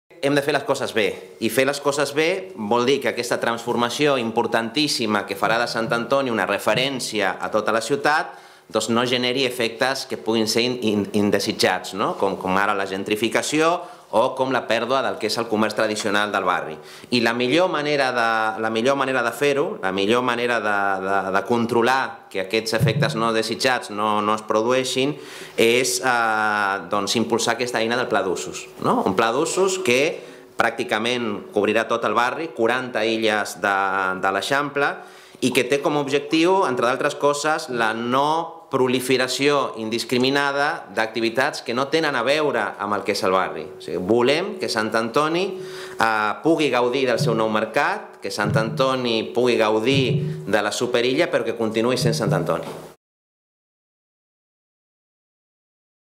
Nou pla d’usos perquè Sant Antoni sigui del veïnat
La rehabilitació del Mercat de Sant Antoni i la superilla marquen l’agenda del futur pla d’usos per limitar l’obertura de nous establiments i comerços que puguin afectar la singularitat del barri.